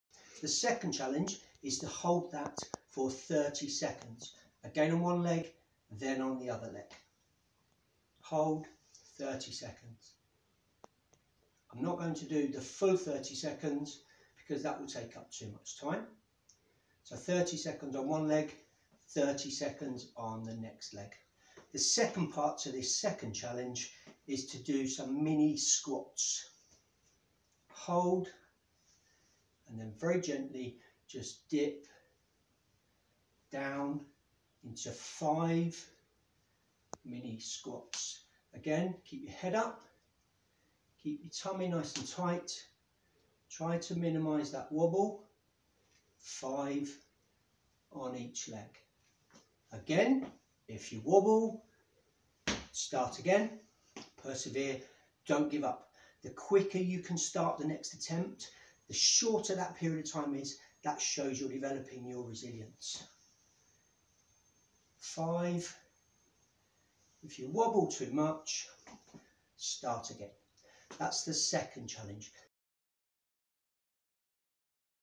Physical Literacy- Static Balance Challenge 2
Introducing our Physical literacy challenges for this week, here is Paul, SDO for @KETTSchoolSport demonstrating a static balance from @Create_Dev